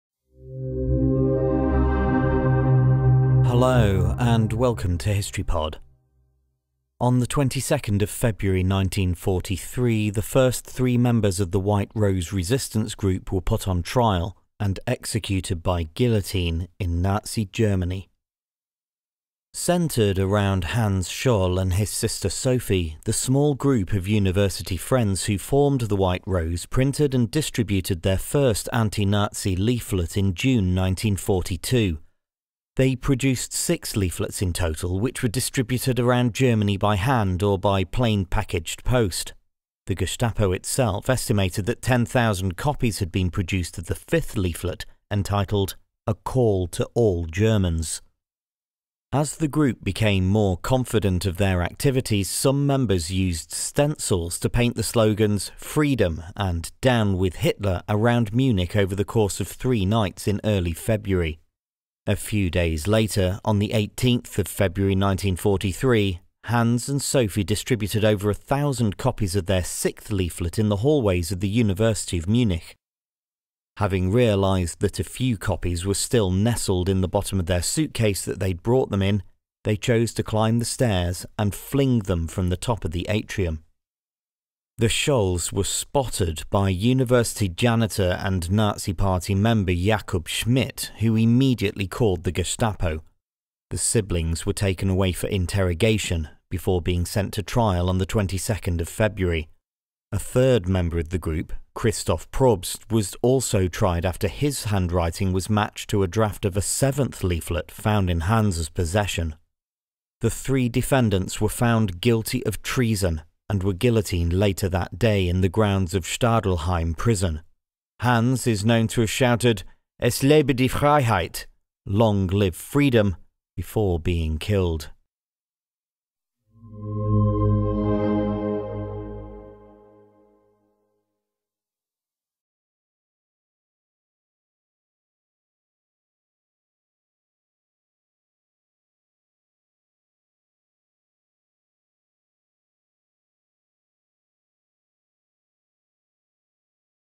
22nd February 1943: Members of the White Rose movement executed by guillotine in Nazi Germany
Centred around Hans Scholl and his sister Sophie, the small group of university friends who formed the White Rose printed and distributed their first anti-Nazi leaflet in June 1942. They produced six leaflets in total, which were distributed around Germany by hand or by plain-packaged post. The Gestapo itself estimated that 10,000 copies had been produced of the fifth leaflet entitled ‘A Call to All Germans!’

As the group became more confident of their activities, some members used stencils to paint the slogans ‘Freedom’ and ‘Down with Hitler’ around Munich over the course of three nights in early February. A few days later, on 18 February 1943, Hans and Sophie distributed over a thousand copies of their sixth leaflet in the hallways of the University of Munich. Having realised that a few copies were still nestled in the bottom of the suitcase they had brought them in, they chose to climb the stairs and fling them from the top of the atrium.

The Scholls were spotted by university janitor and Nazi Party member Jakub Schmid who immediately called the Gestapo. The siblings were taken away for interrogation before being sent to trial on 22 February. A third member of the group, Christoph Probst, was also tried after his handwriting was matched to a draft of a seventh leaflet found in Hans’ possession.

The three defendants were found guilty of treason and were guillotined later that day in the grounds of Stadelheim Prison. Hans is known to have shouted ‘Es lebe die Freiheit! - Long live freedom!’ before being killed.